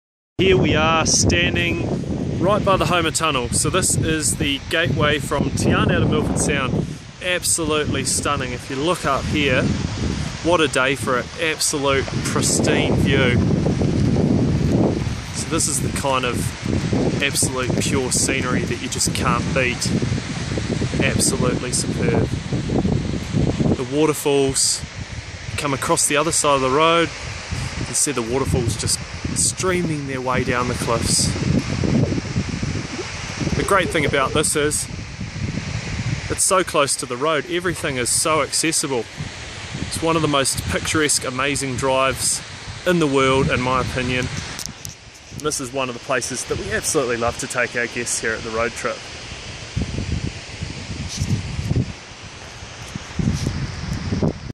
The Picturesque Milford Road
The amazingly picturesque Milford Road. One of our absolute favourite drives. This video is shot near the Homer Tunnel, just before the crossing through to Milford Sound from Te Anau